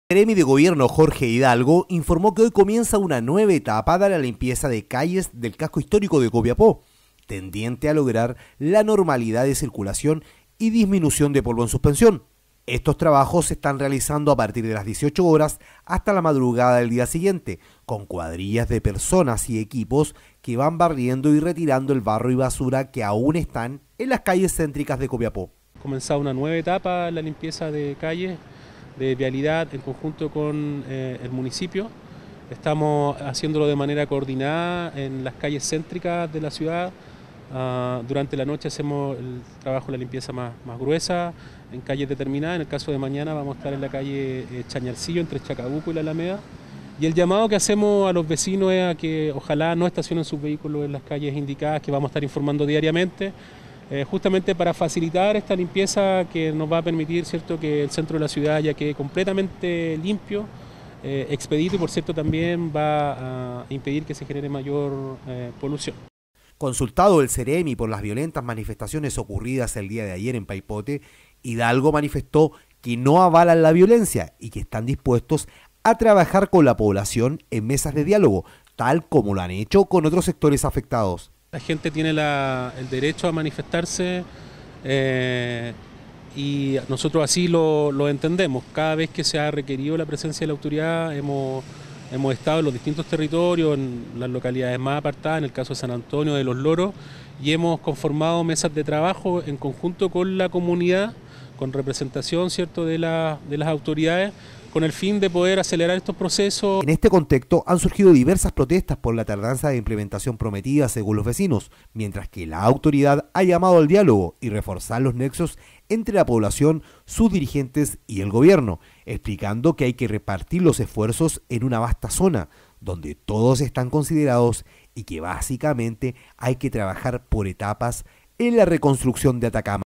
Vecinos de Atacama protestan por incumplimiento de compromisos tras aluvión
Siguen los trabajos de normalización en Atacama, luego que se registraran diversas manifestaciones por el incumplimiento de compromisos, según denunciaron vecinos afectados.